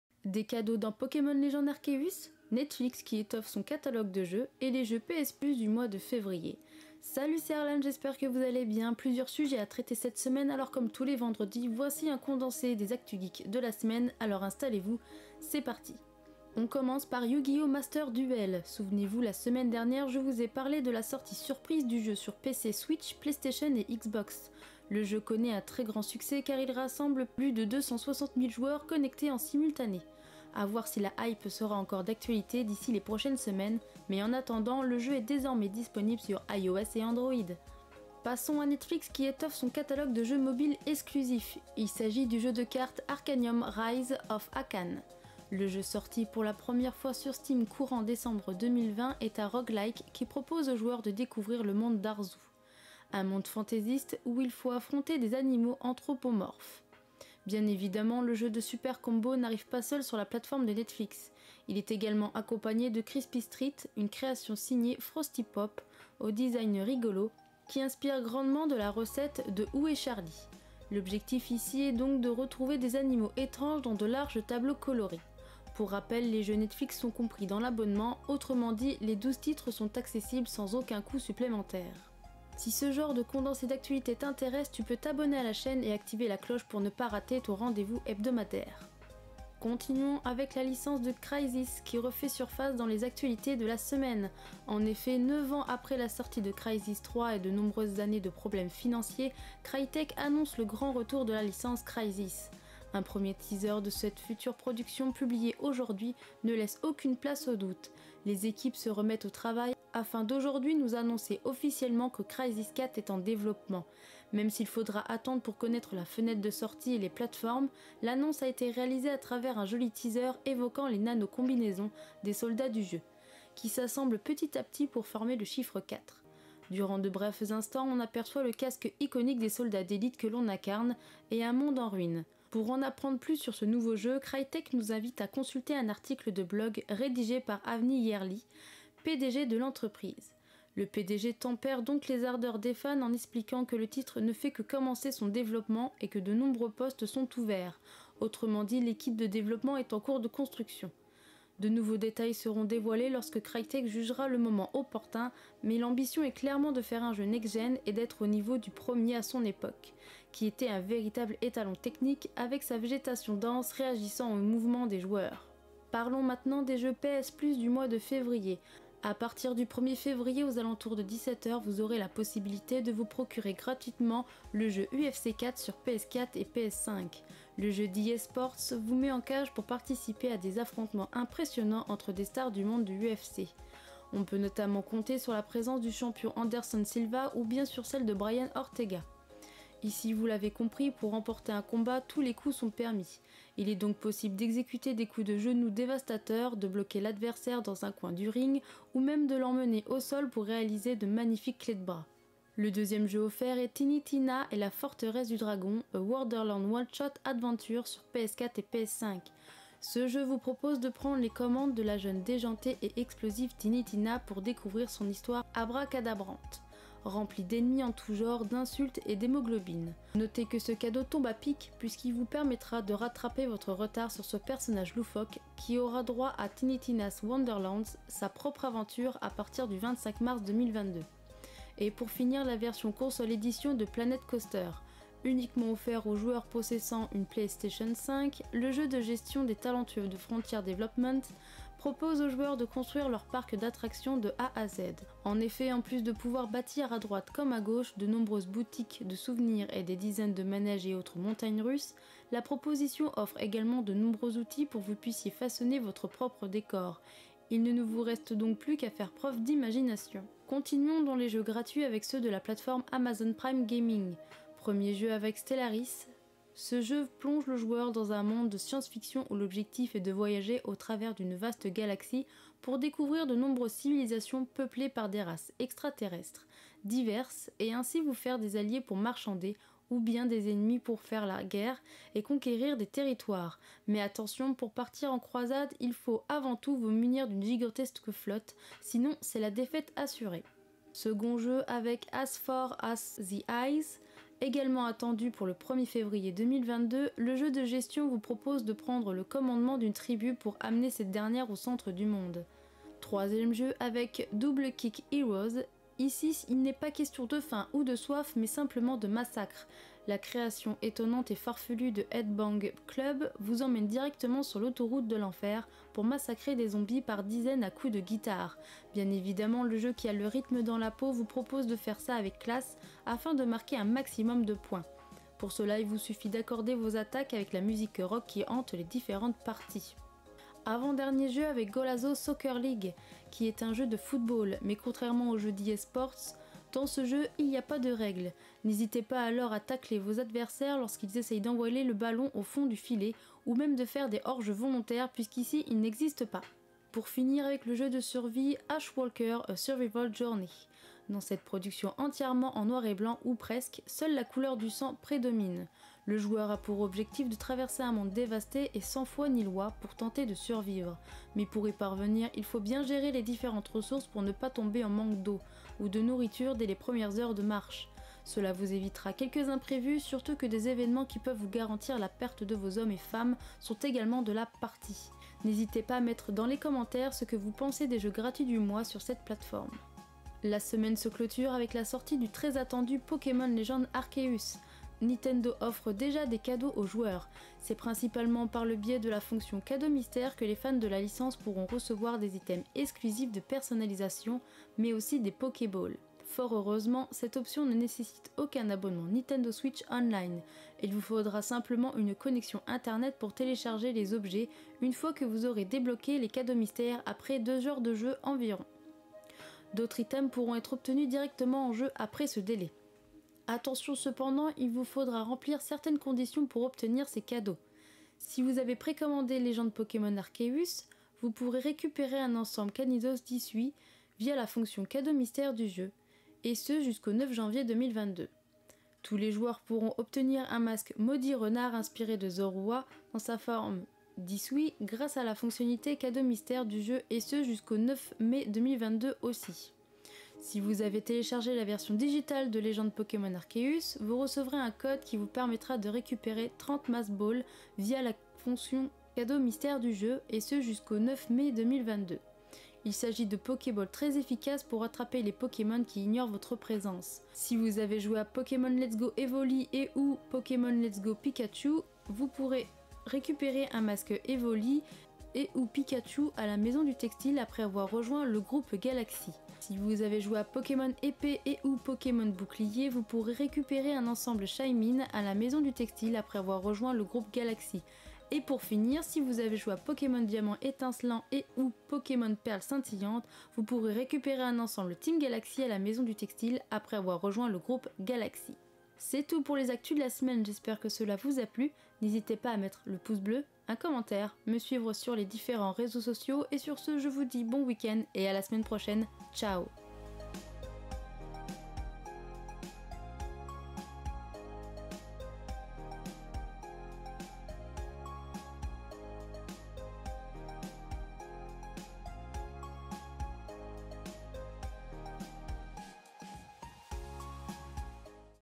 Cadeaux dans Légendes Pokémon: Arceus, les jeux PS+ de Février et Netflix ajoute des jeux mobiles
Chapitres:
00:00 Intro
00:16 Un record pour Yu-Gi-Oh Master Duel
00:38 Netflix ajoute des jeux mobiles dans son catalogue
01:29 Crytek tease Crysis 4
02:45 Jeux gratuits PS+ Février 2022
04:20 Jeux gratuits Amazon Prime Gaming Février 2022
06:20 Des cadeaux pour les joueurs de Légendes Pokémon: Arceus
08:08 Outro

Je monte mes vidéos avec Adobe Première Pro et filme avec un canon EOS M10